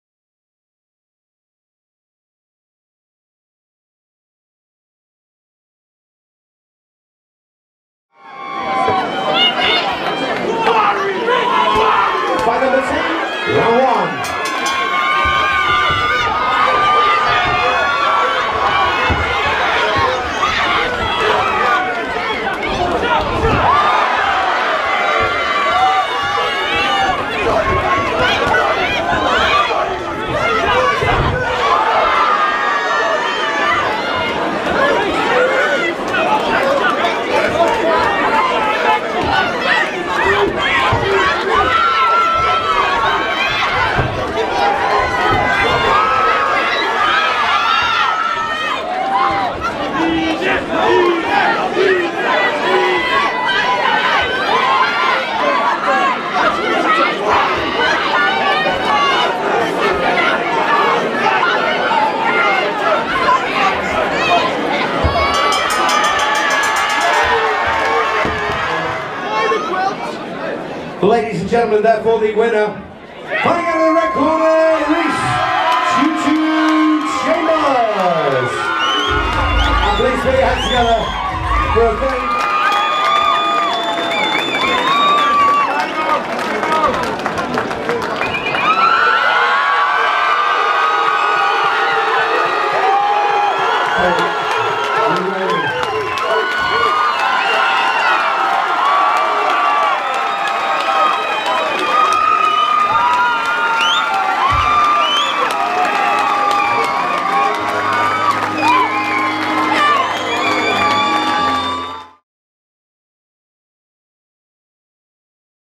Ultra White Collar Boxing | Wolverhampton | Reece Chambers VS James Gwilt
Wolverhampton Ultra White Collar Boxing 3rd December 2017

8 Weeks FREE training / Get in the best shape of your life/ Raise money for Cancer Research UK